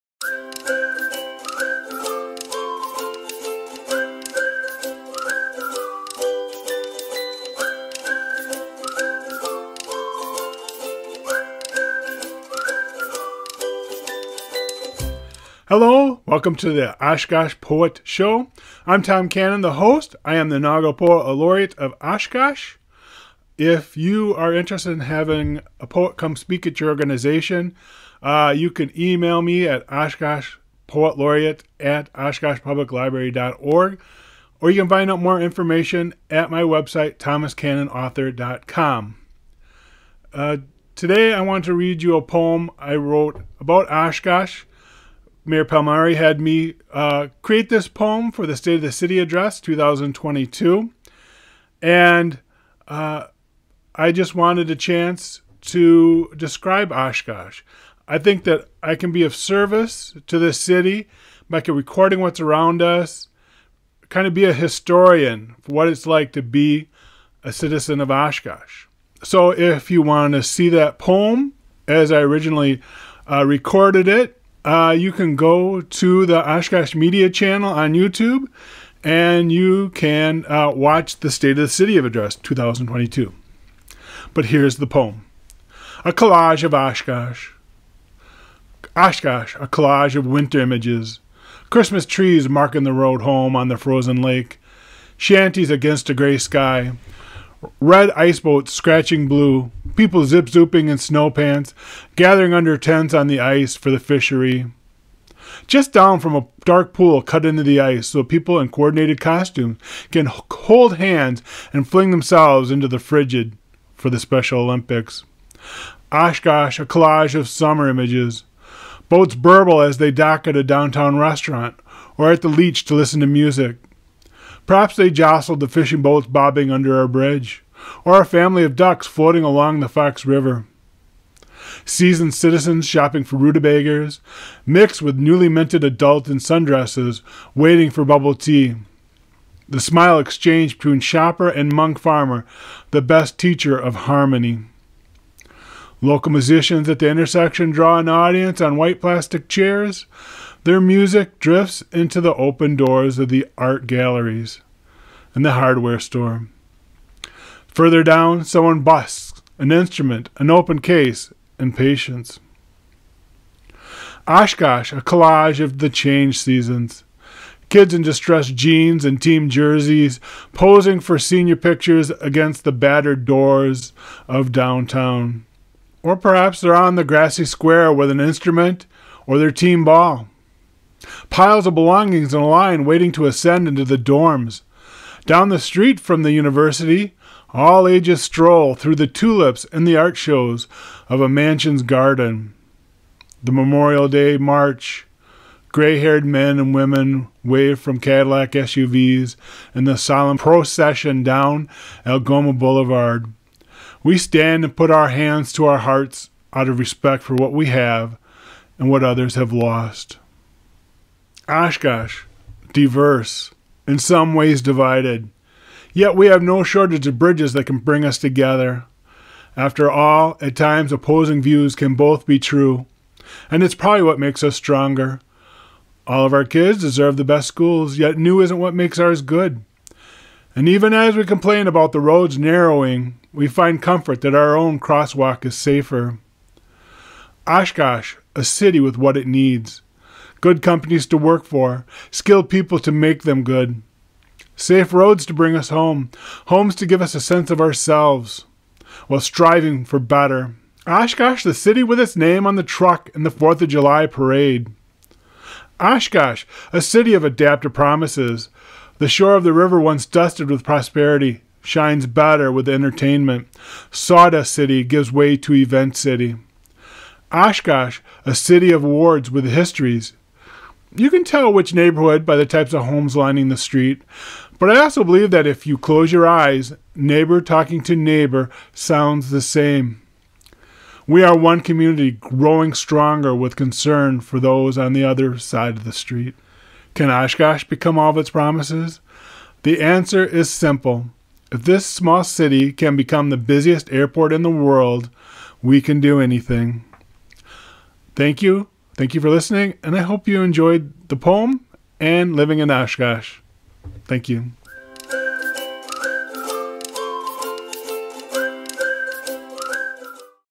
Oshkosh Poet Show -The Collage of Oshkosh
Tom reads the poem he wrote for Oshkosh's State of the City address.  City Manager Mark Rohloff and the Oshkosh Media Department created an engaging presentation on the future of Oshkosh.

For more information on the poet laureate program and Tom Cannon's books, please go to thomascannonauthor.com